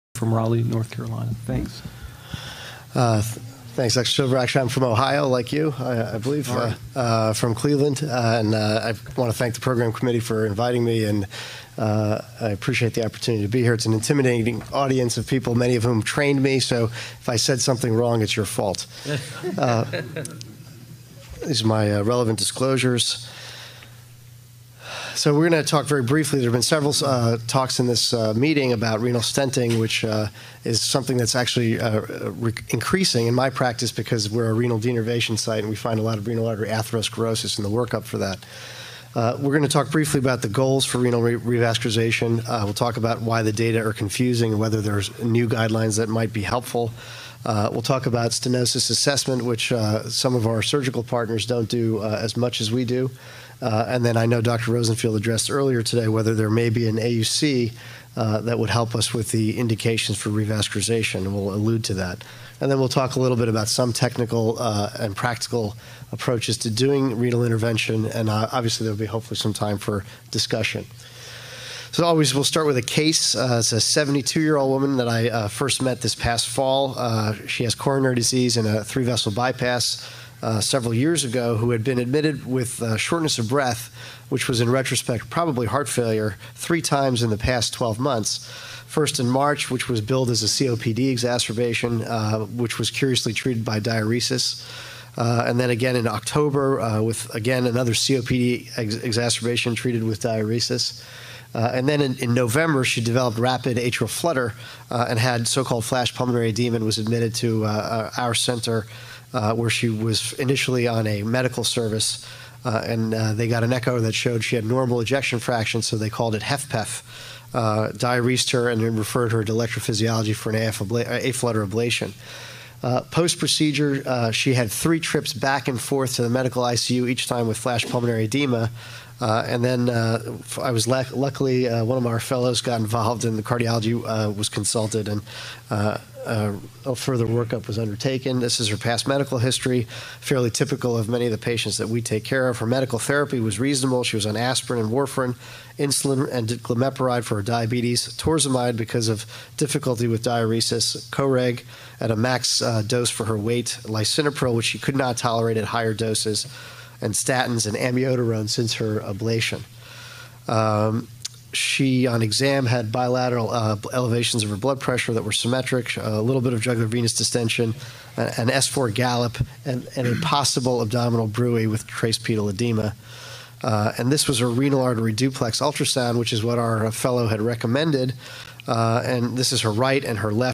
Renal Artery Stenting: Patient Selection and Technique
Sahil A. Parikh, MD
Harrington Heart & Vascular Institute
Cleveland, OH, USA

Presented at SCAI 2013

to watch the complete video and find other clinical videos